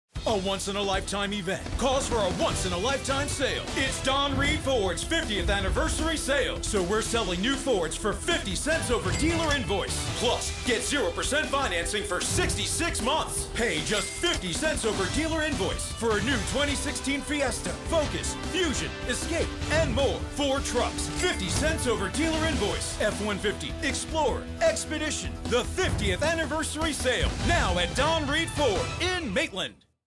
Don Reid Ford's 50th Anniversary 50 Cent Over Invoice Sale
Celebrating our 50th Year making you our #1 priority. Central Florida's Oldest Family Owned Ford Dealership.